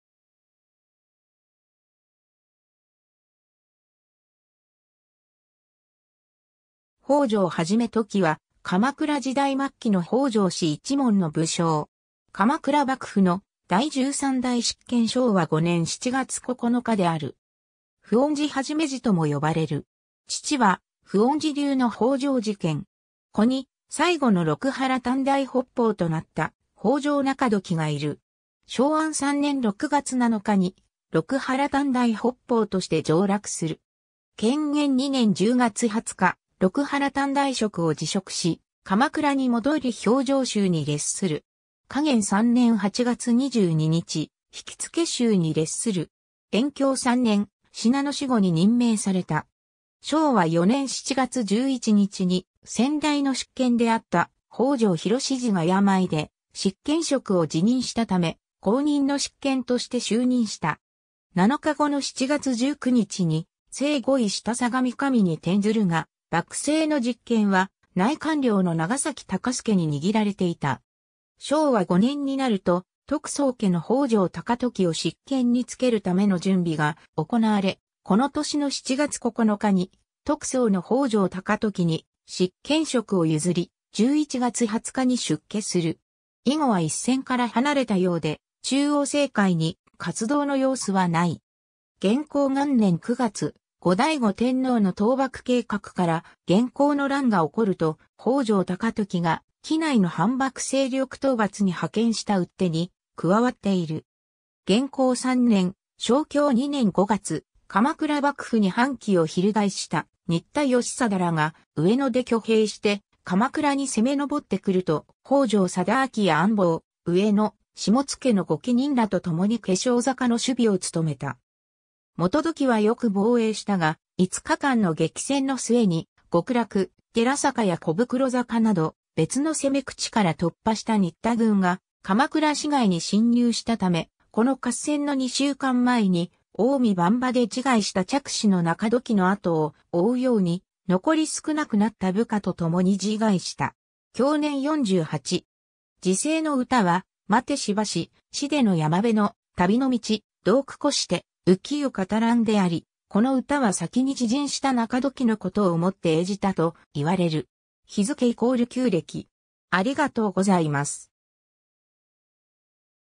北条基時
北条氏_(普恩寺流)
北条 基時（ほうじょう もととき）は、鎌倉時代末期の北条氏一門の武将。
鎌倉幕府の第13代執権（在職：正和4年7月11日（1315年8月11日） - 正和5年7月9日（1316年7月28日））である。
普恩寺 基時（ふおんじ もととき）とも呼ばれる。
 父は普恩寺流の北条時兼。
子に最後の六波羅探題北方となった北条仲時がいる。
 正安3年（1301年）6月7日に六波羅探題北方として上洛する。
乾元2年（1303年）10月20日、六波羅探題職を辞職し、鎌倉に戻り評定衆に列する（評定衆になっていない説もある）。
嘉元3年（1305年）8月22日、引付衆に列する。
延慶3年（1310年）、信濃守護に任命された。
 正和4年（1315年）7月11日に先代の執権であった北条煕時が病で執権職を辞任したため、後任の執権として就任した（煕時とは7代執権北条政村を曽祖父に持つ点で共通しており、血縁的にはとこの関係にあった）。
7日後の7月19日に正五位下相模守に転ずるが、幕政の実権は内管領の長崎高資に握られていた。
 正和5年（1316年）になると得宗家の北条高時を執権に就けるための準備が行なわれ、この年の7月9日に得宗の北条高時に執権職を譲り、11月20日に出家する。
以後は一線から離れたようで中央政界に活動の様子は無い。
 元弘元年（1331年）9月、後醍醐天皇の倒幕計画から元弘の乱が起こると、北条高時が畿内の反幕勢力討伐に派遣した討手に加わっている。
 元弘3年/正慶2年（1333年）5月、鎌倉幕府に反旗を翻した新田義貞らが上野で挙兵して鎌倉に攻め上ってくると、北条貞顕や安房・上野・下野の御家人らと共に化粧坂の守備を務めた。
基時はよく防衛したが、5日間の激戦の末に極楽寺坂や巨福呂坂など別の攻め口から突破した新田軍が鎌倉市街に侵入したため、この合戦の2週間前に近江番場で自害した嫡子の仲時の後を追うように、残り少なくなった部下と共に自害した。
享年48。
 辞世の歌は「待てしばし、死出の山辺の、旅の道、同く越て、浮世語らん」であり、この歌は先に自刃した仲時の事を思って詠じたと言われる。
 ※ 日付＝旧暦